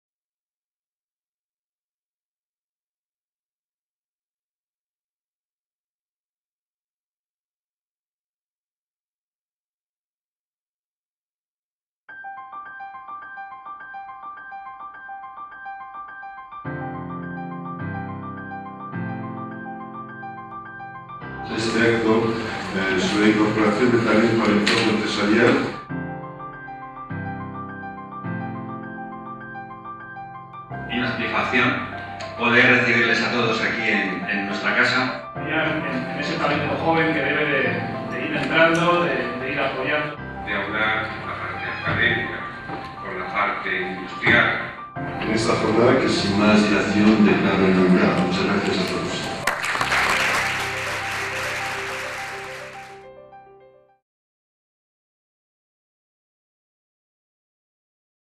Jornada 'Incorporación de talento al entorno empresarial'
Resumen de la inauguración de la Jornada 'Incorporación de talento al entorno empresarial: nuevas convocatorias de ayudas', celebrada el 06 de marzo de 2015 en el Salón de Actos de la Facultad de Veterinaria de la Universidad Complutense de Madrid.